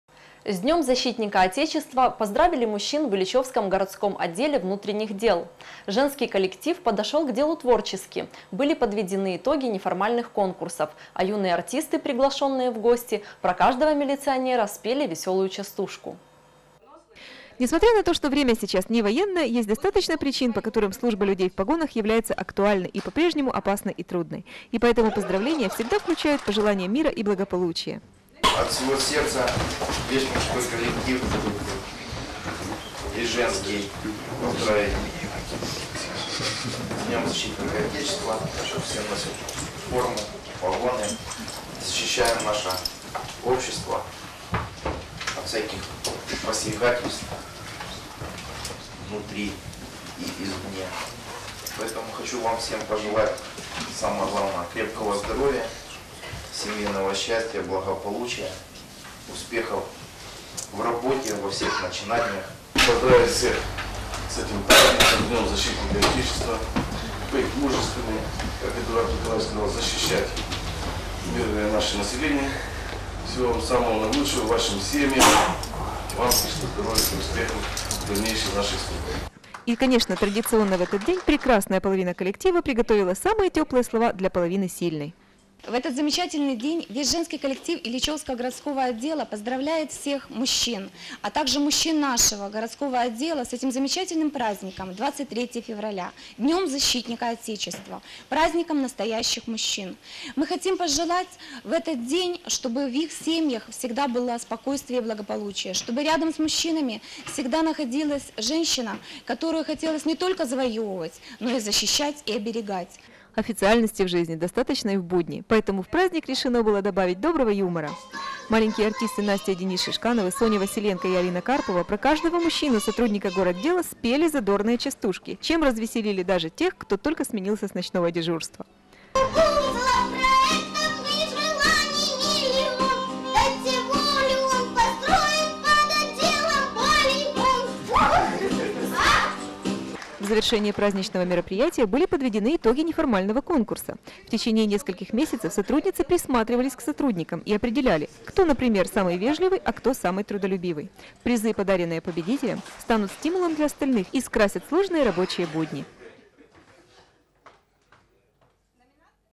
День защитника Отечества в милиции
С Днем защитника отечества поздравляли мужчин и в Ильичевском городском отделе внутренних дел. Женский коллектив подошел к делу творчески, были подведены итоги неформальных конкурсов, а юные артисты, приглашенные в гости, про каждого милиционера спели веселую частушку.